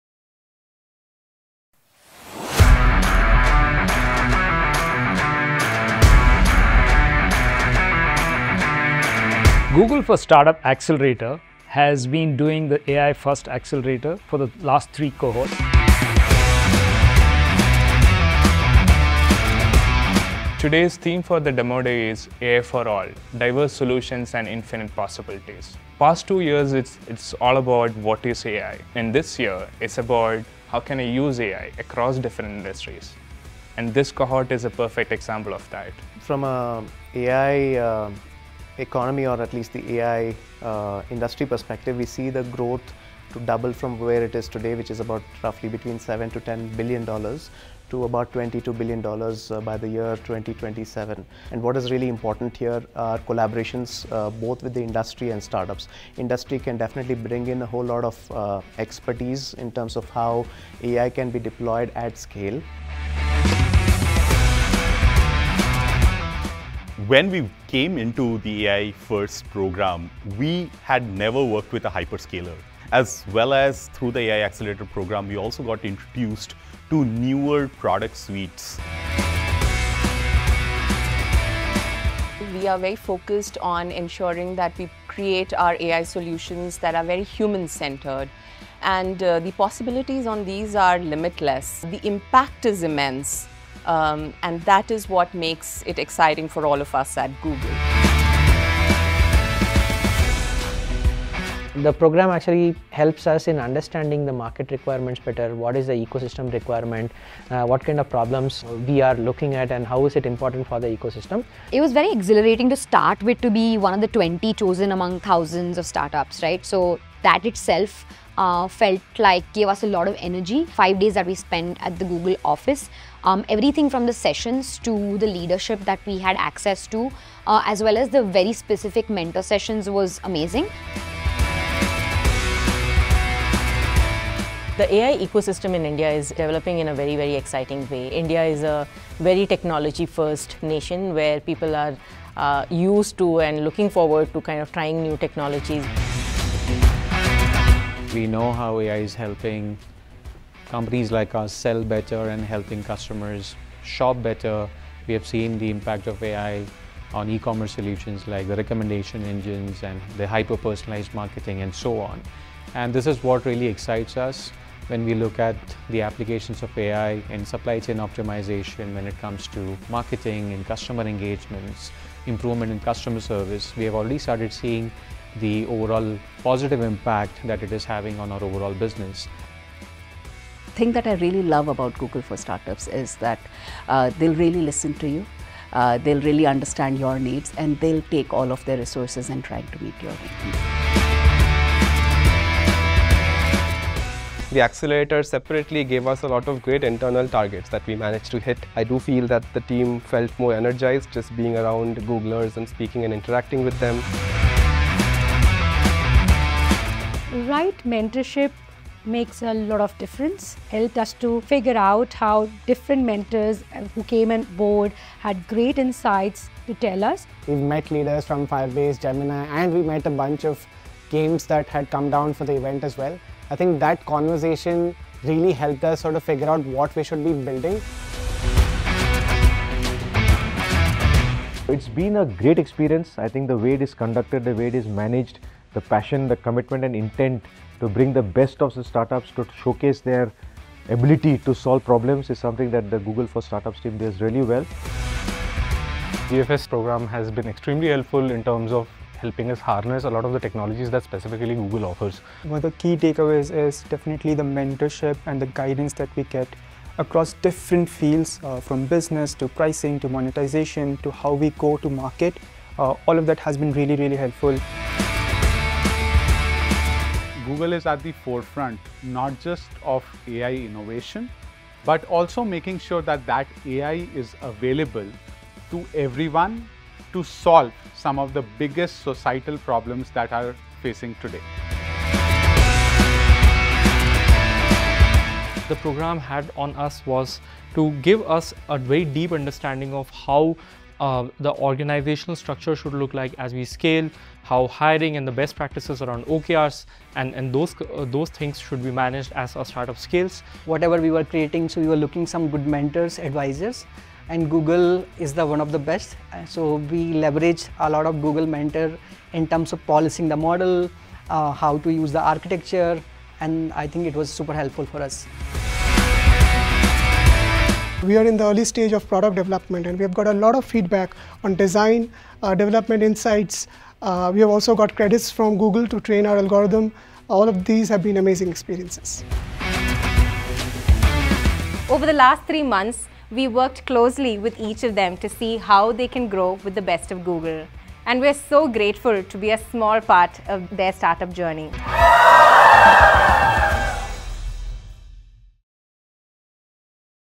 Google for Startups Accelerator: AI First (India) 2024 Class Demo Day Highlights
Google for Startups Accelerator: AI First (India) provides a three-month, equity-free program for Indian AI First startups between Seed to Series A. Launched in July 2024, the program began with a week-long in-person bootcamp and continued with virtual support tailored to each startup's objectives and key results (OKRs). This support included one-on-one mentorship, networking opportunities, training, and more.

Watch highlights from the 2024 Class Demo Day, where 20 AI-first startups pitched their businesses and showcased live demos to investors, Googlers, and other startup ecosystem enablers at an in-person event in Bengaluru, India, followed by a networking high-tea.